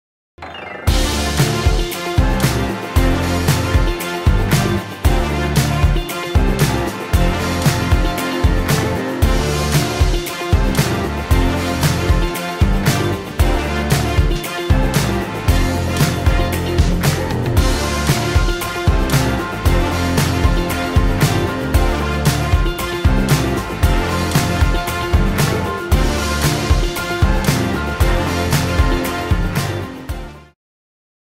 Happy Hour at Sea Captain's House | SeaCaptains.com
Ease into the hour with fried fish bites or Bloody Mary Clams. Our Beef satay tickles the taste buds as you throw a cold one down the hatch! Enjoy all this alongside an breathtaking oceanfront view at Myrtle Beach’s Sea Captain’s House – voted the Best Seafood Restaurant four years in a row.